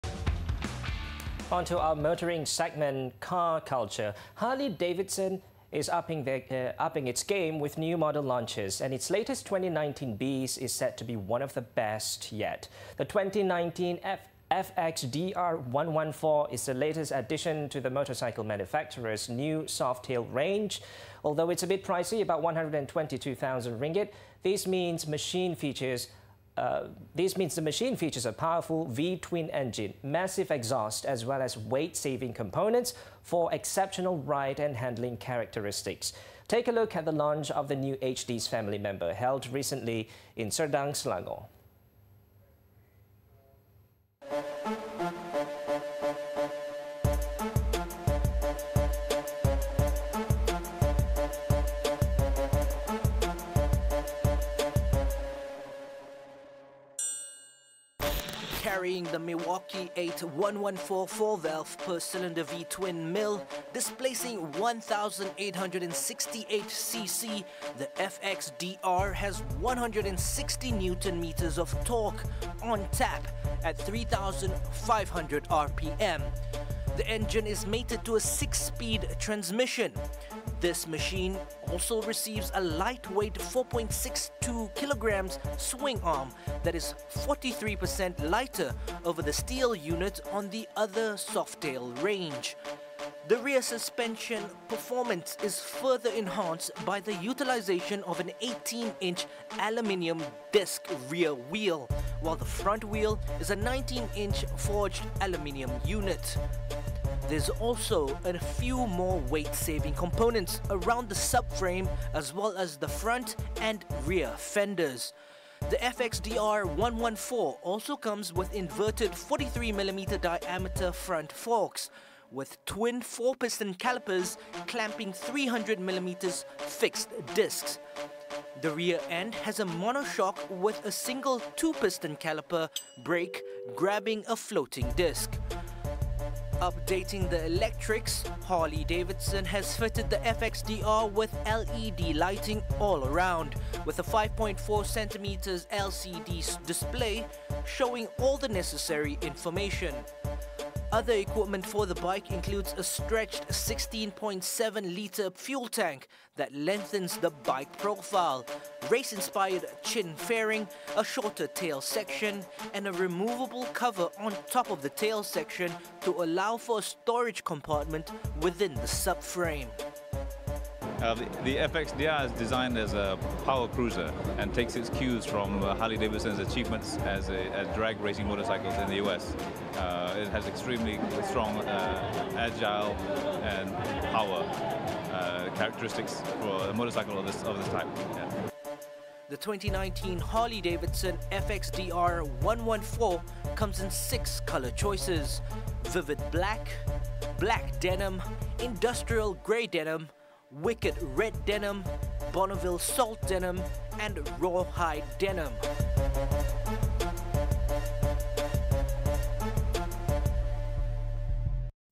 Power Cruiser | Harley Davidson Introduces Drag-Racing Inspired FXDR 114
Harley-Davidson is upping their game, with new model launches and their latest 2019 beast is said to be one of the best yet.